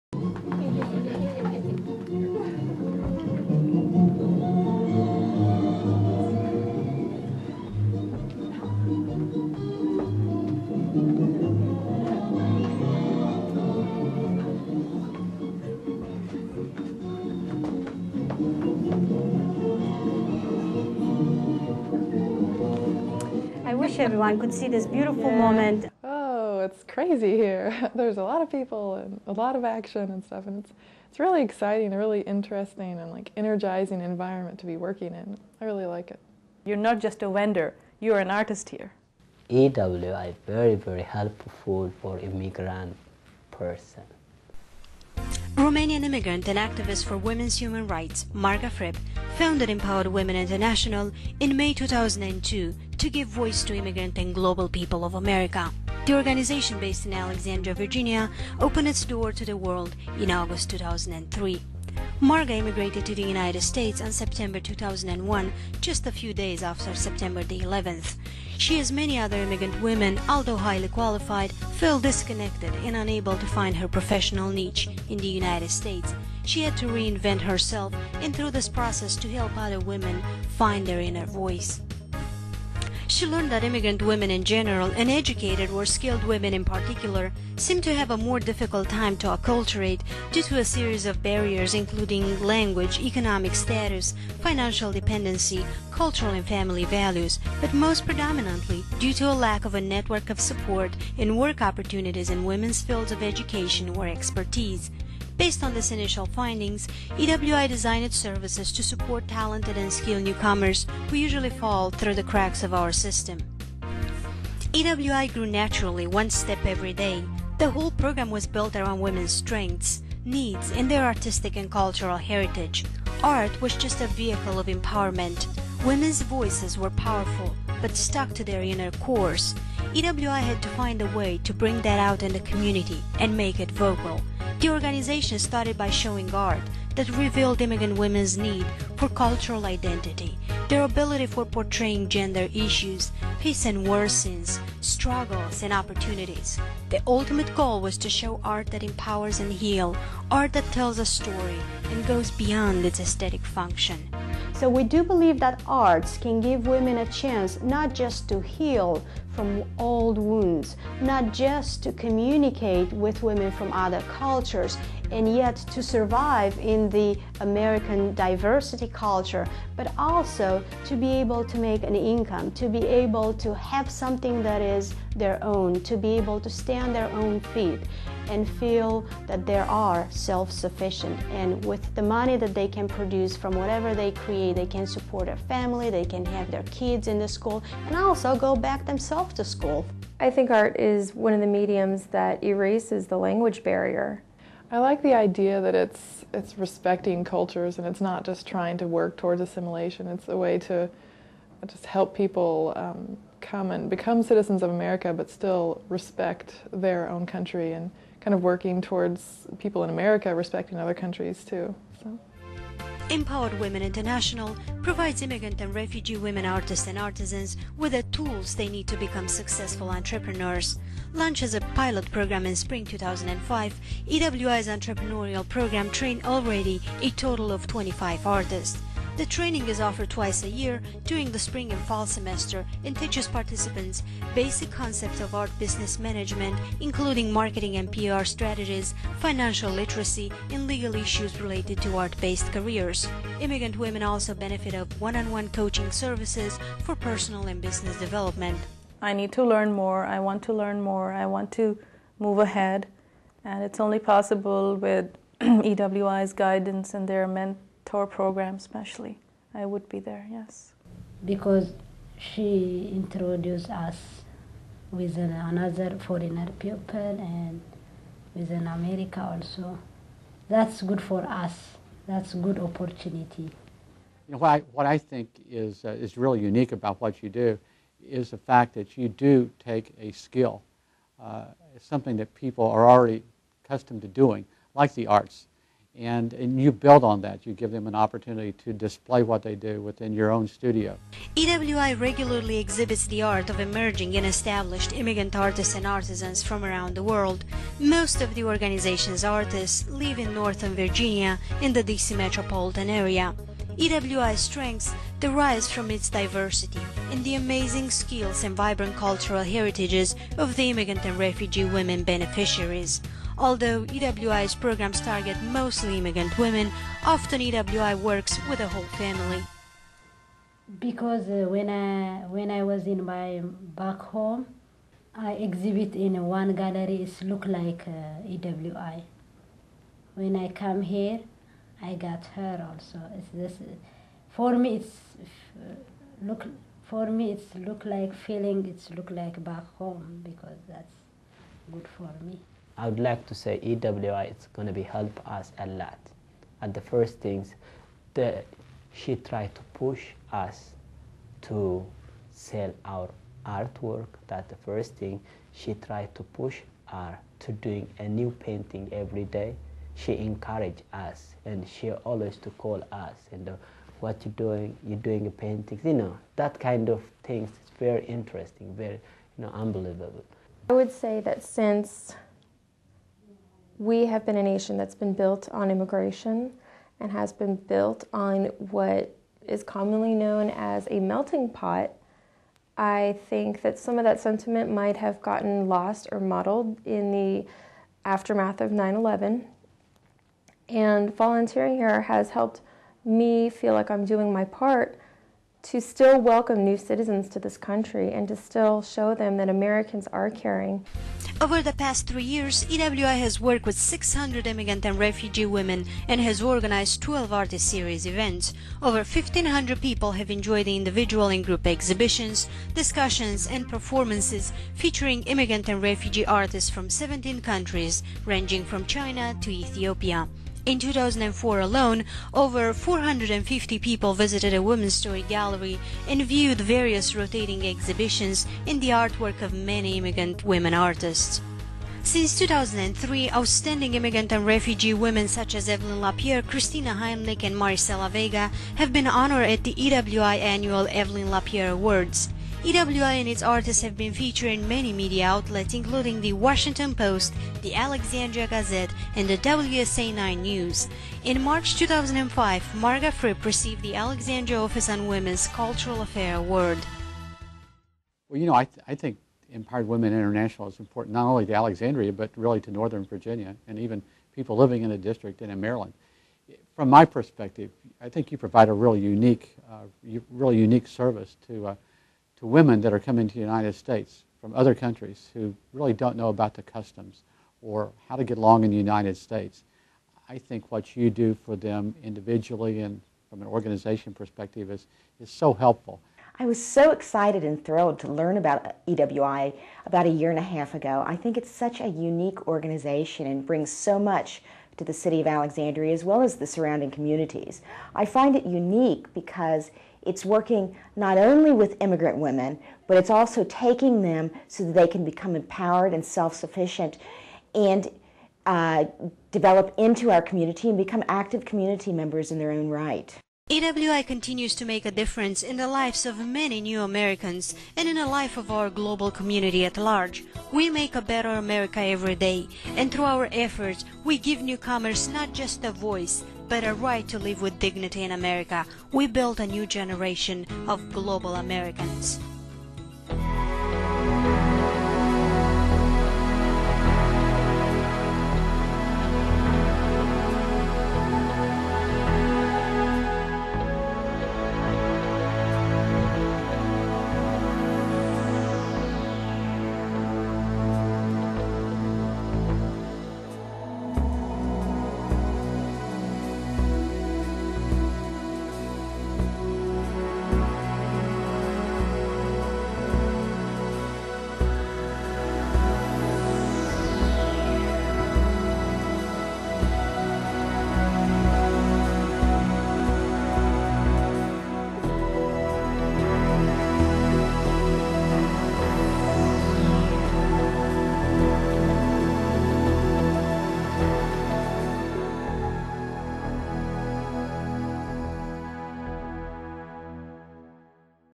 The Journey to America
The Journey to America:Pathways to Immigration Through the Arts, highlights Empowered Women International's outreach, educational and business development programs in Alexandria, VA. Marga C.Fripp, founder and Executive Director of the nonprofit EWI, narrates.